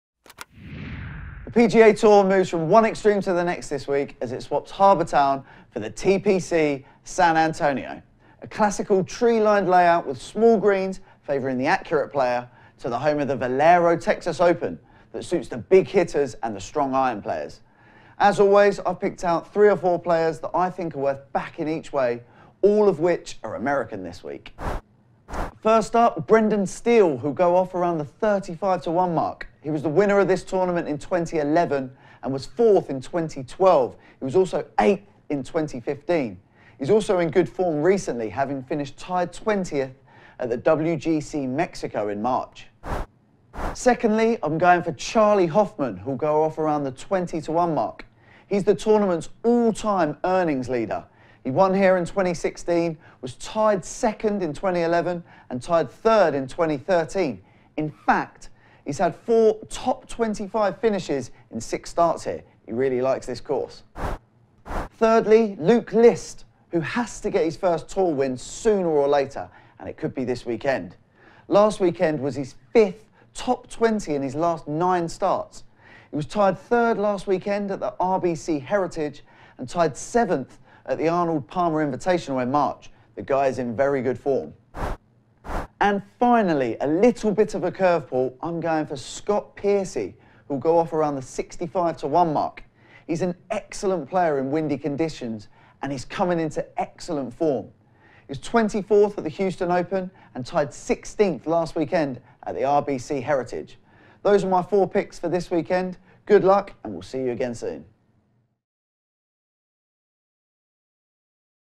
Valero Texas Open - Betting Tips |19th-22nd April
Here are our betting tips for the Valero Texas Open.